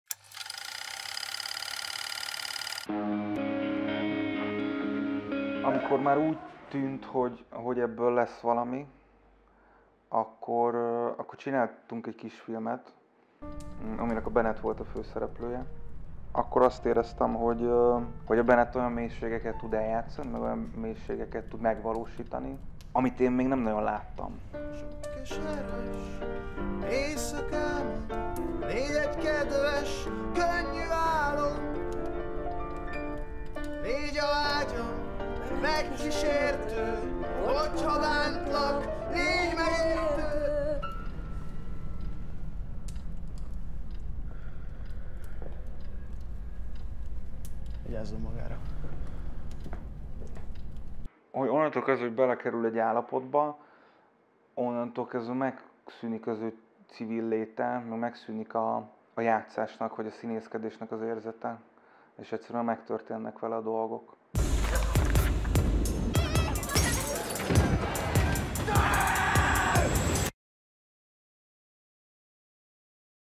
Mozinet Intro: Miért Vilmányi Benett film főszereplője?
Nézd meg a filmet a Cinegón!

A juhászként tengődő, 21 éves Ádám (Vilmányi Benett) magának való vidéki fiú, aki szégyelli dadogását. Feszült, mégis meghitt hidegségben él egykor erőszakos, mára a vallásban menedéket lelt és megszelídült édesapjával (Thuróczy Szabolcs). Ádám egészen addig nem találja a helyét, amíg rá nem ébred, hogy a rapzene segítségével – Larry művésznéven – kifejezheti magát, és szembe nézhet a tehetetlenségből fakadó frusztrációival. Explicit, dühtől fűtött szövegeire felfigyelnek a helyi fiatalok, majd a neten váratlan mértékű népszerűségre tesz szert. Egy országos tehetségkutató lehet számára a kitörési lehetőség, ám ehhez előbb a kezébe kell vennie a saját élete feletti irányítást.

Bernáth Szilárd erőteljes bemutatkozó rendezése fiatalokat megszólító, társadalmilag érzékeny film Vilmányi Benett és Thuróczy Szabolcs döbbenetesen hiteles alakításával.